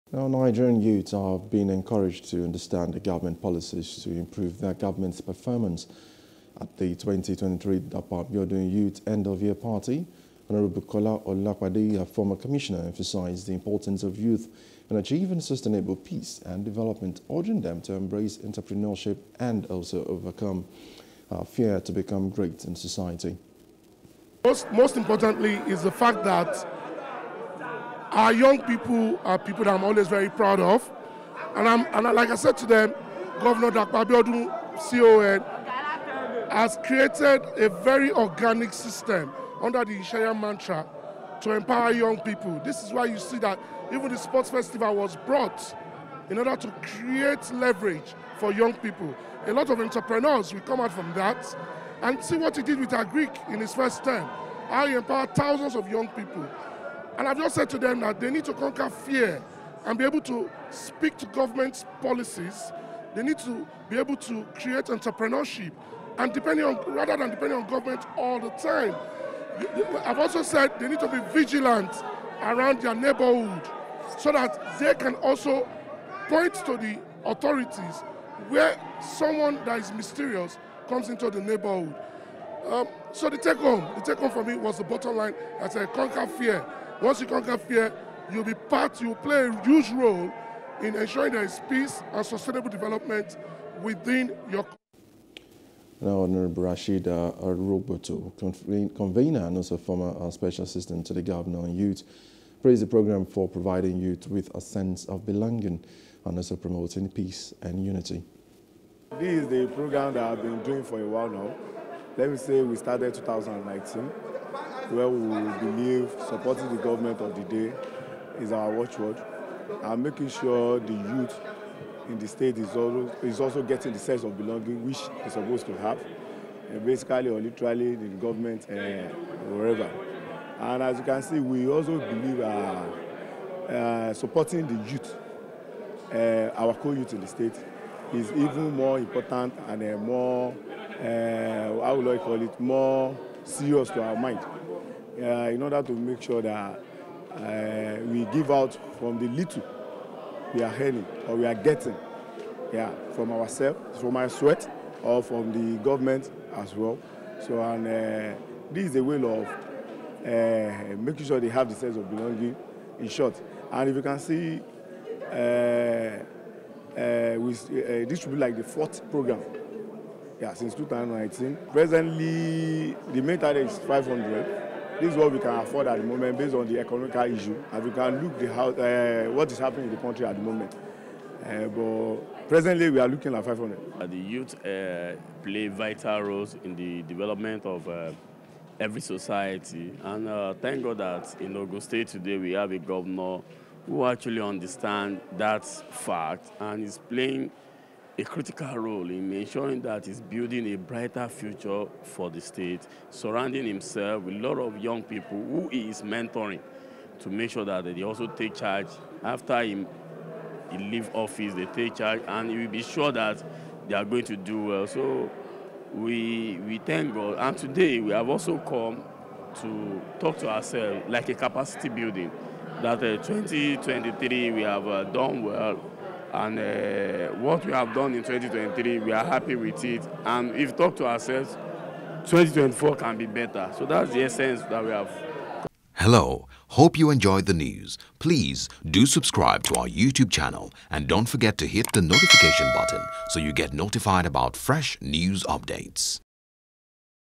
Governance: Youths Challenged On Understanding Government Policies
Nigerian youths are encouraged to understand government policies to improve their government's performance.